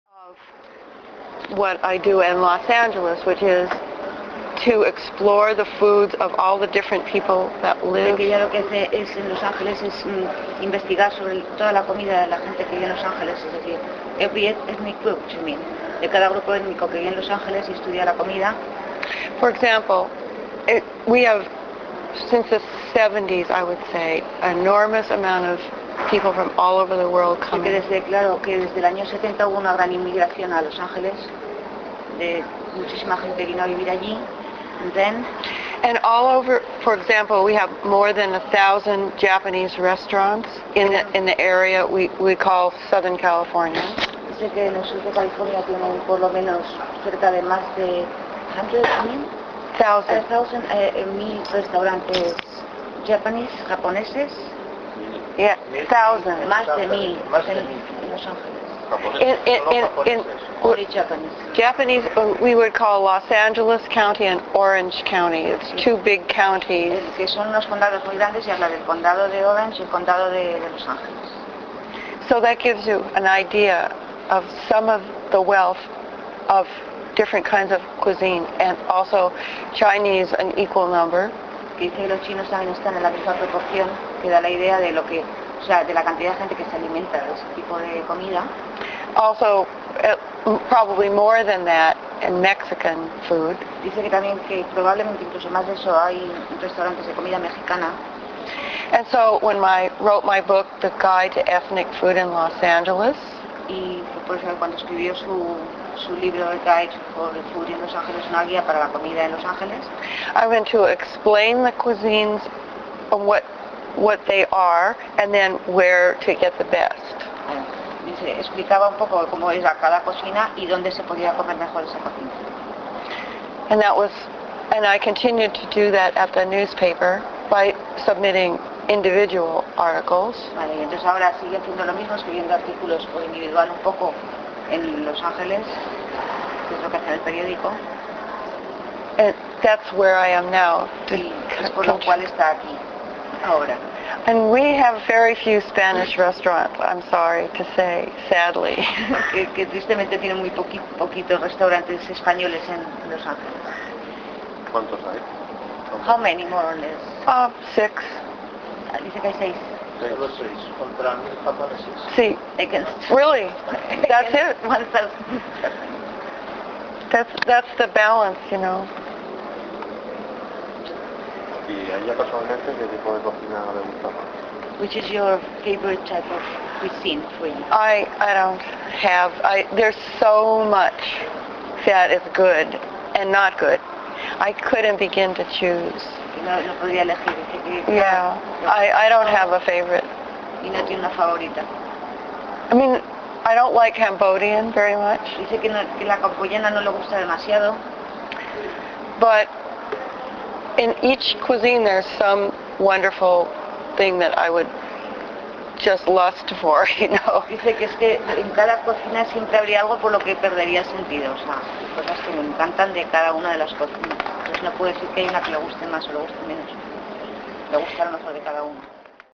Linda Burum
Linda Burum es especialista en gastronomía internacional. Ha escrito varios libros: Eat 2011 Los Angeles: The Food Lover?s Guide to Los Angeles, A Guide to Ethnic Food in Los Angeles: Restaurants, Markets, Bakeries, Specialty Shops for the Food of Cambodia, China, Ethiopia, Greece, Guatemala,Asian Pasta: A Cook's Guide to the Noodles, Wrappers, and Pasta Creations of the East, etc... Escribe habitualmente en Los Angeles Time y otras revistas especializadas.
En octubre del 2010 viaja por España descubriendo su cocina